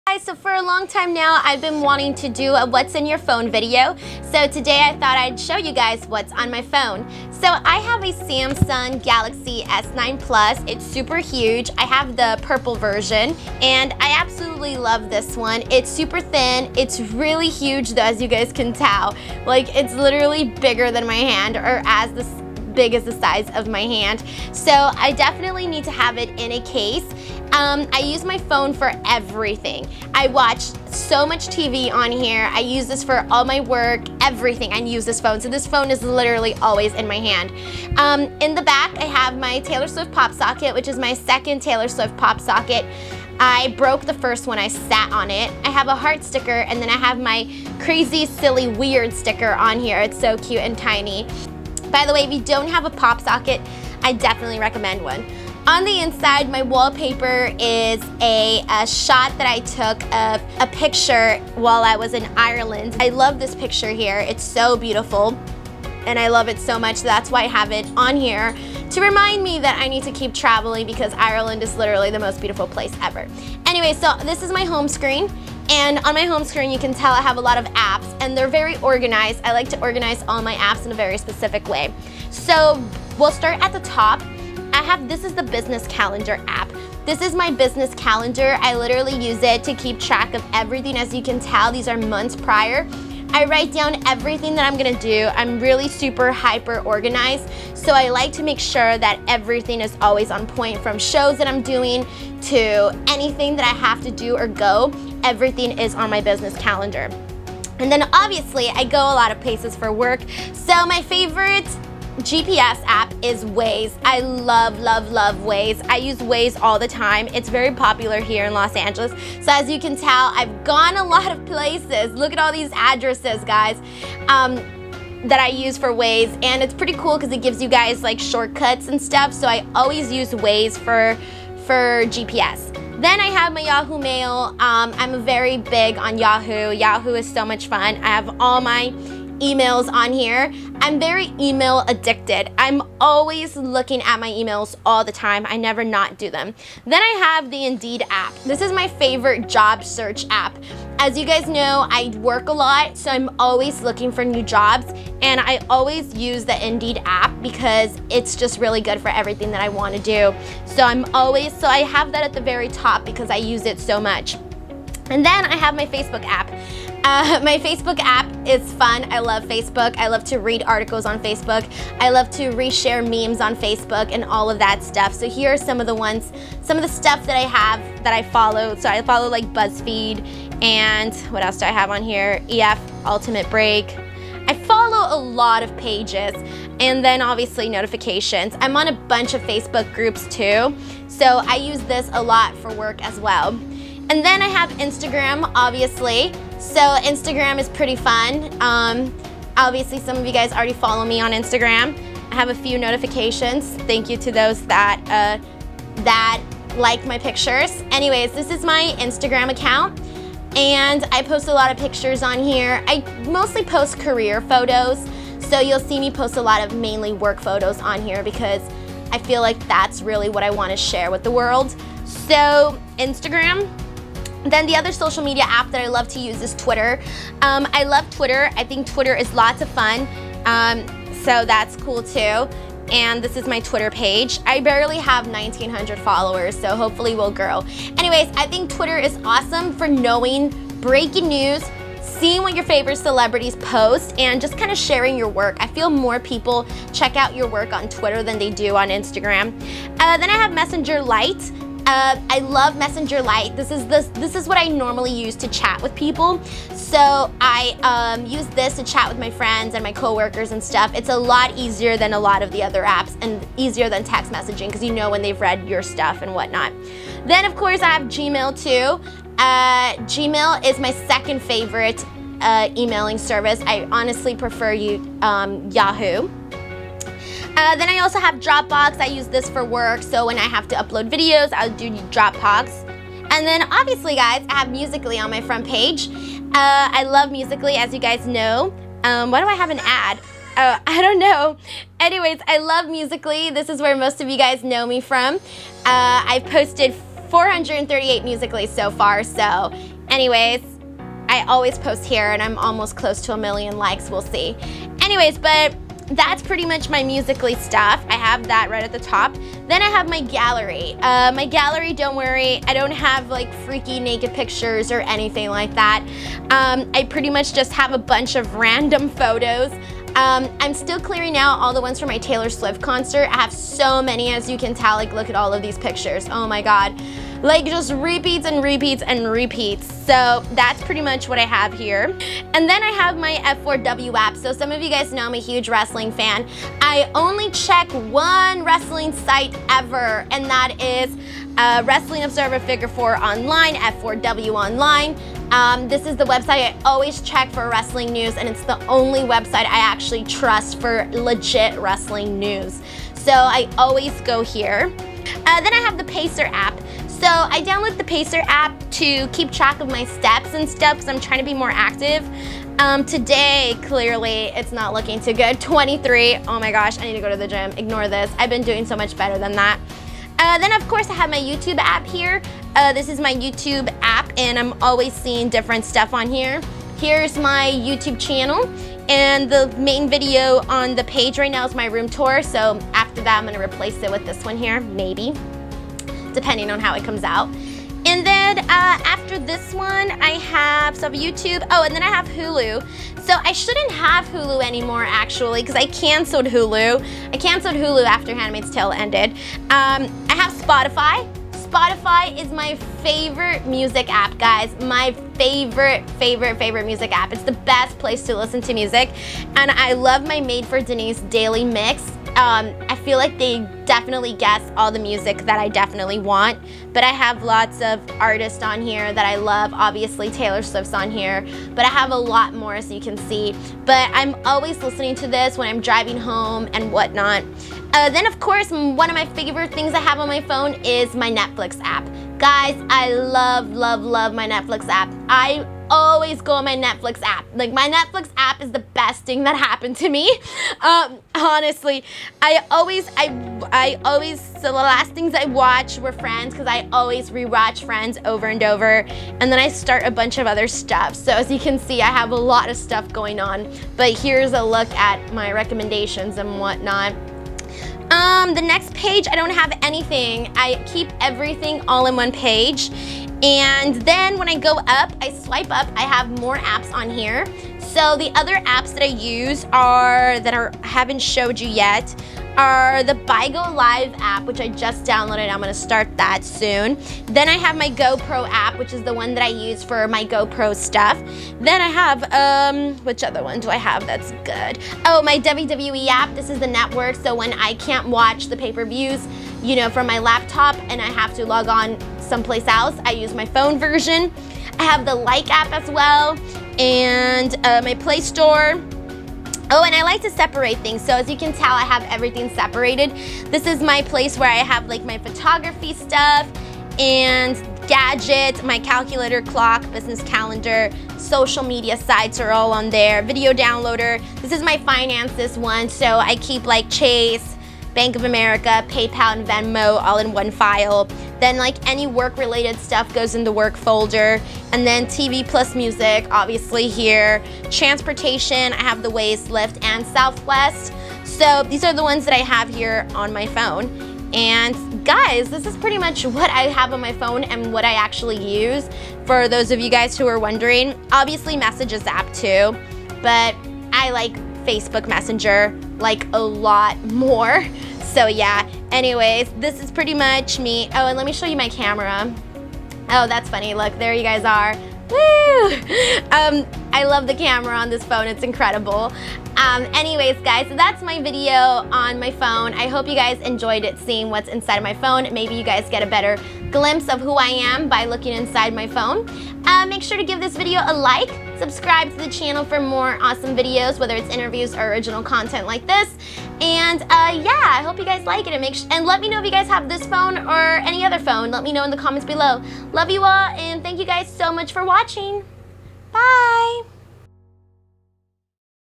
What's On My Phone | Samsung Galaxy S9 Plus!
I've been wanting to do this video for quite sometime now! I love my new SamsungGalaxyS9Plus ! It's so awesome and so I am sharing with you all my favorite apps, gallery and pretty much everything that I have on my phone!

I hope you all enjoy this special look into my phone!